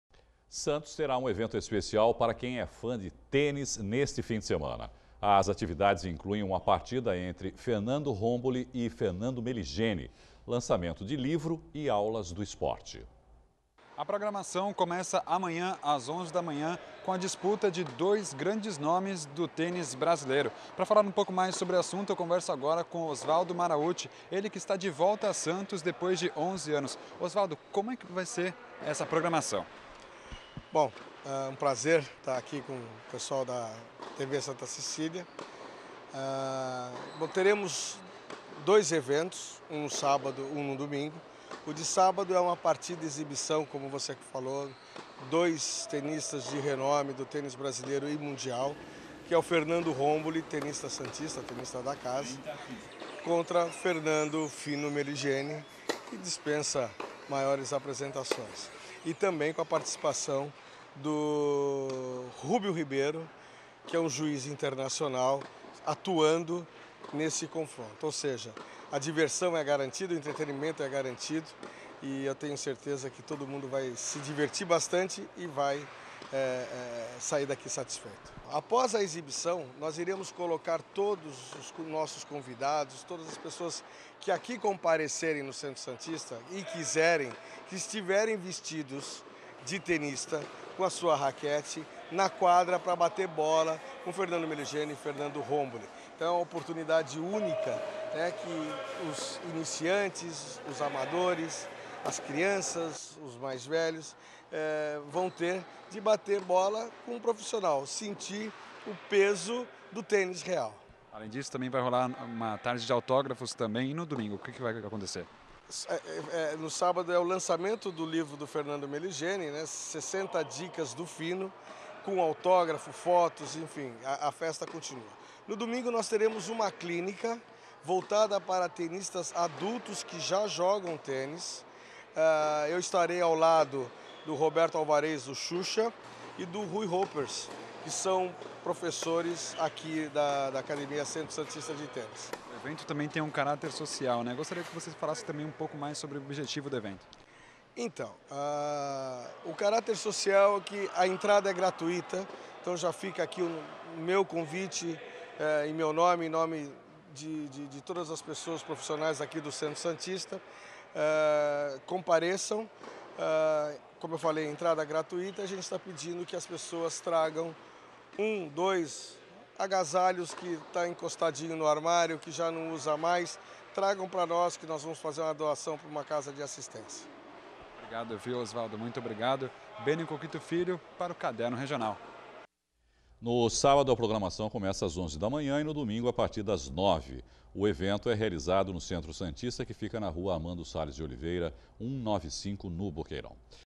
Santos recebe evento especial de tênis
Santos terá um evento especial de tênis neste final de semana. As atividades incluem a partida entre Fernando Romboli e Fernando Meligeni, lançamento de livro e aulas do esporte, no Centro Santista de Tênis.